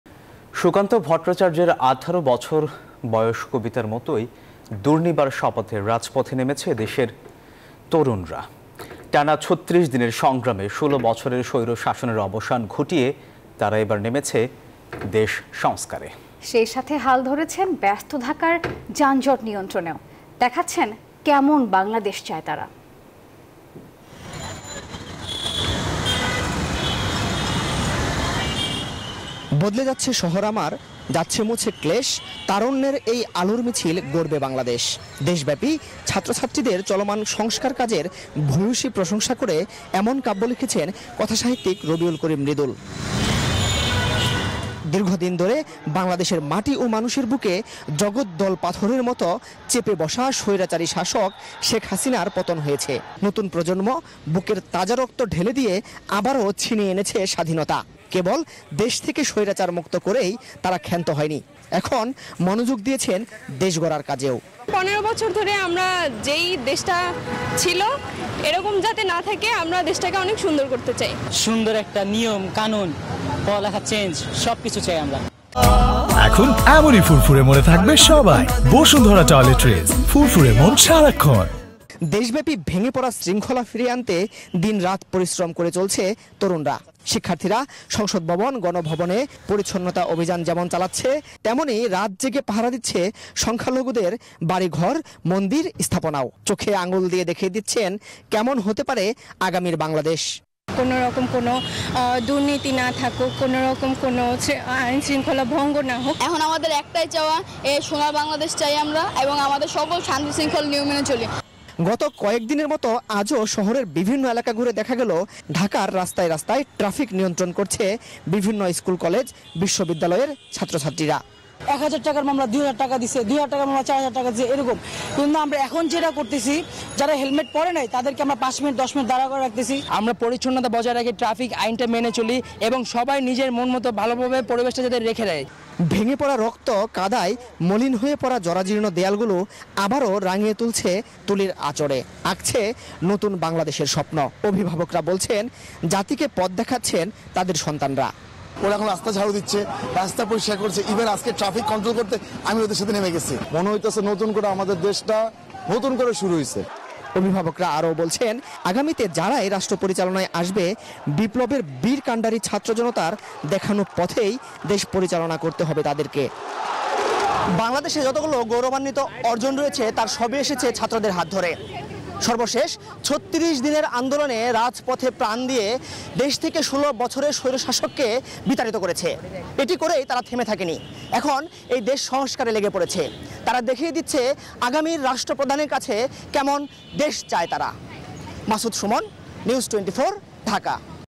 ঢাকার ট্রাফিক নিয়ন্ত্রণে কাজ করছে শিক্ষার্থীরা | News24
NEWS24 Team